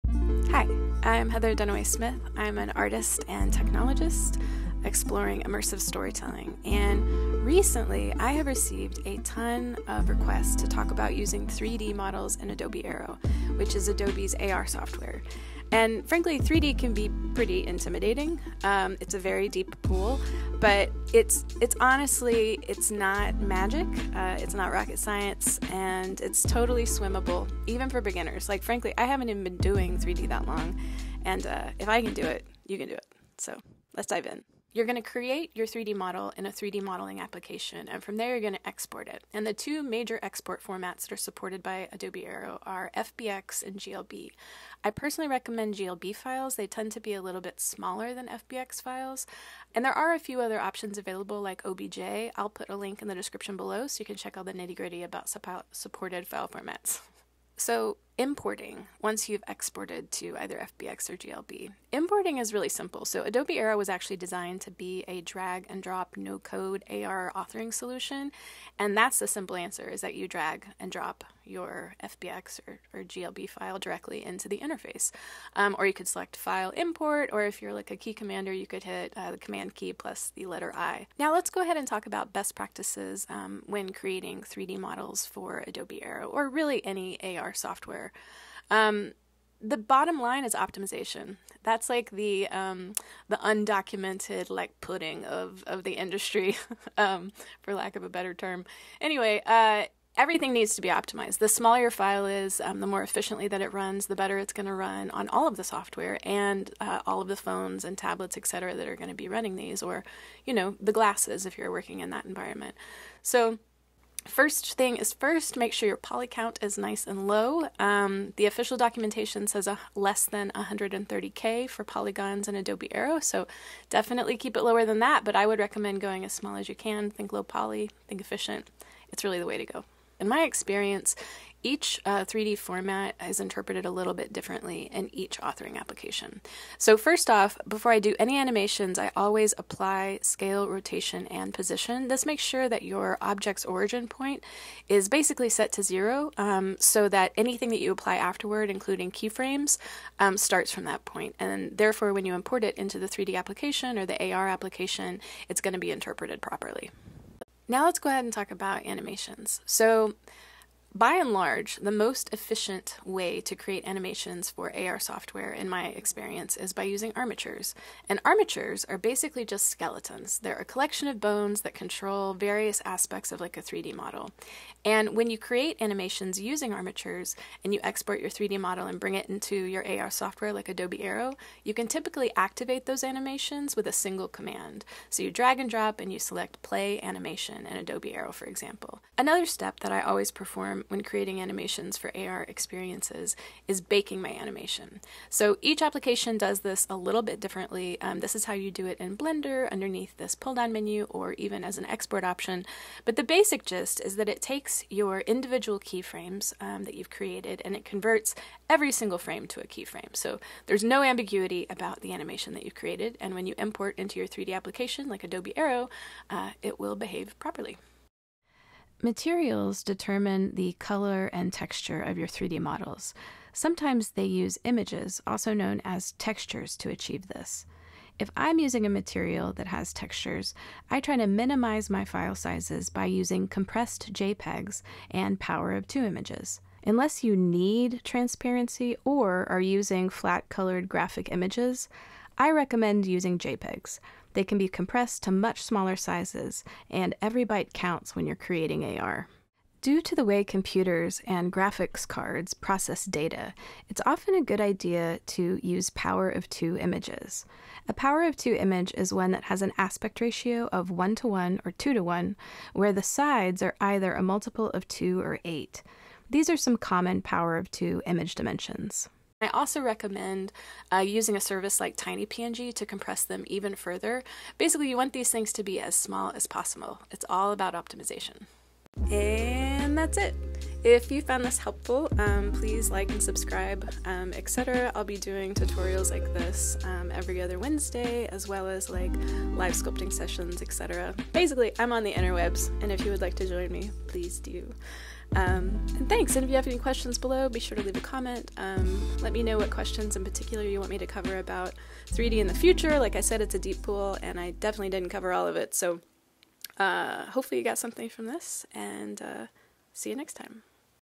How to use 3D in AR using Adobe Aero
Recently, I’ve been getting a lot of questions asking how to use 3d objects in Adobe Aero: how to import them, what file types are supported, etc. So, here is an overview of 3D best practices and techniques. I hope it helps!

Read more about all the supported file types on the official Adobe Aero